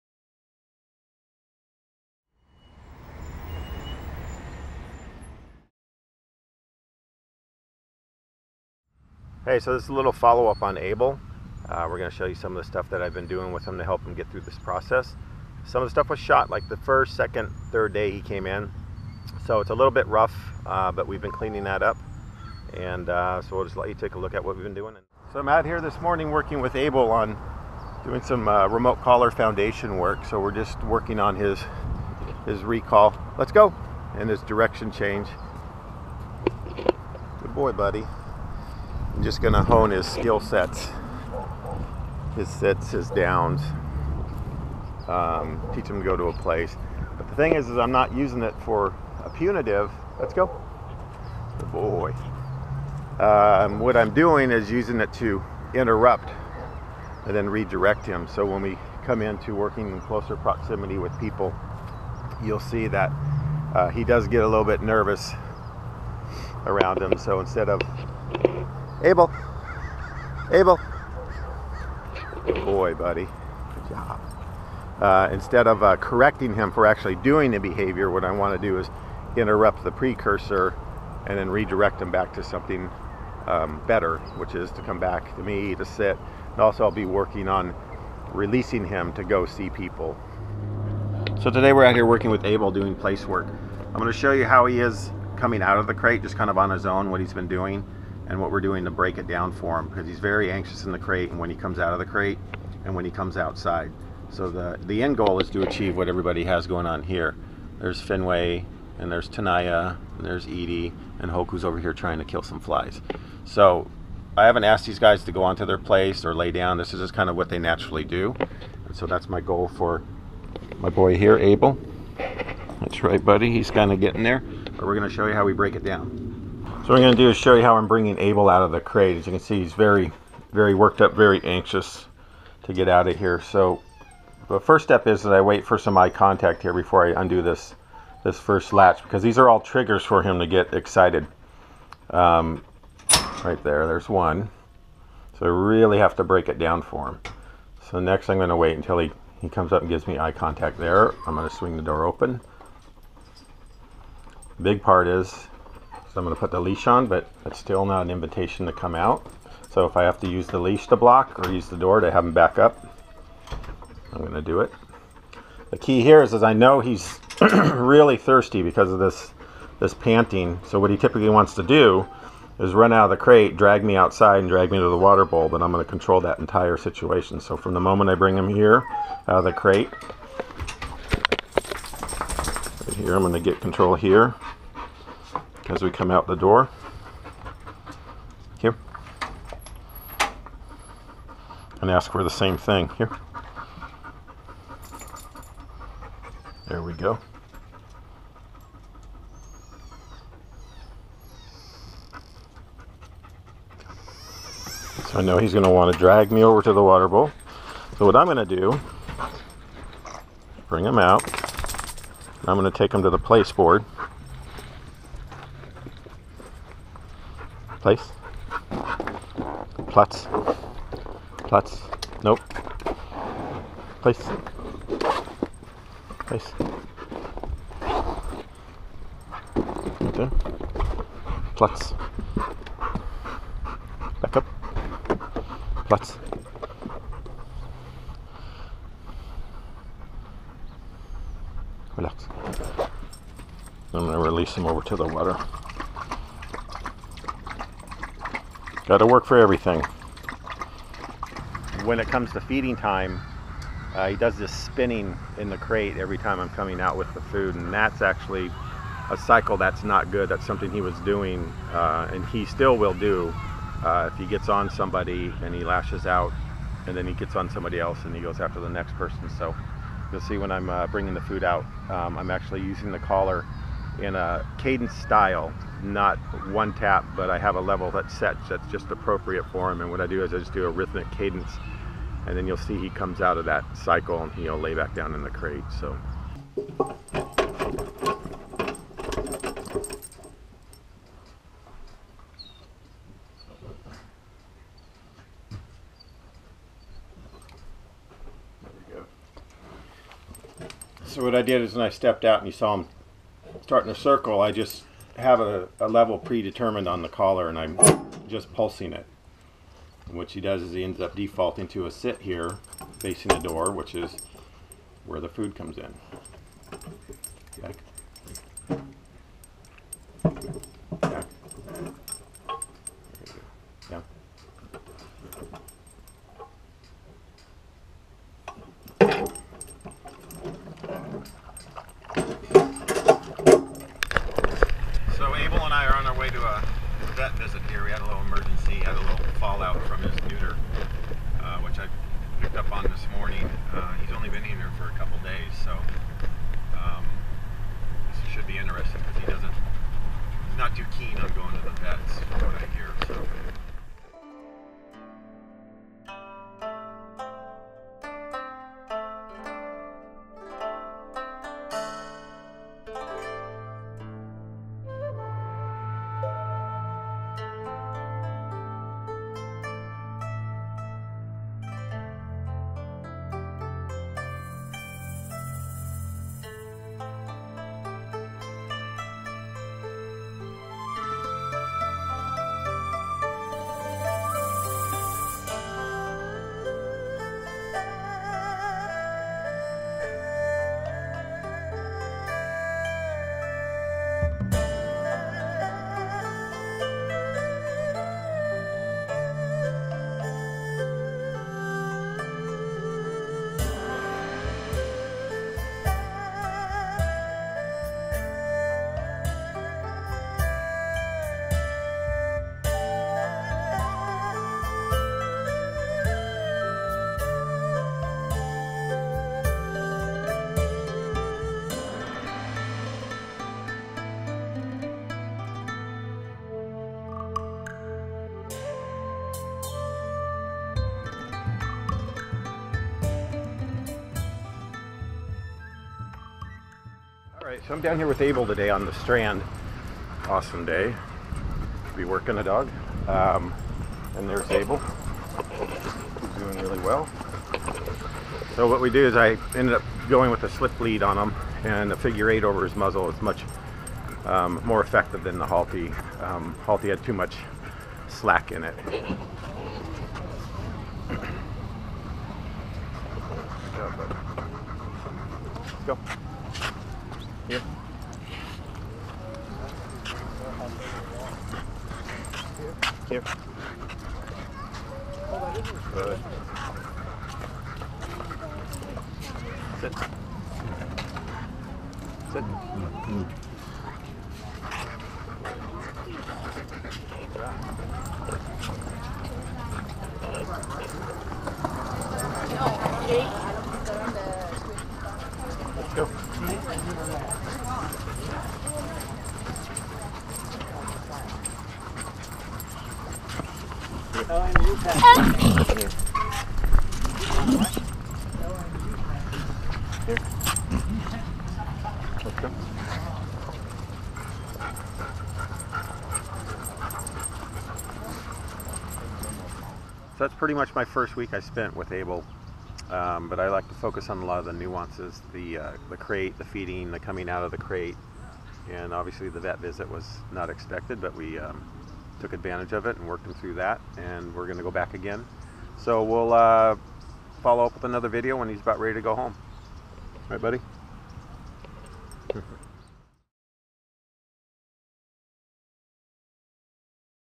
Able Follow Up
Able came in with some "human aggression" issues. All of his so called aggression is a by product of him being fearful and anxious. Once these to issues are addressed then the side effect, the aggression, will go away. Building plenty of trust with a dog like this is what allows us to help him.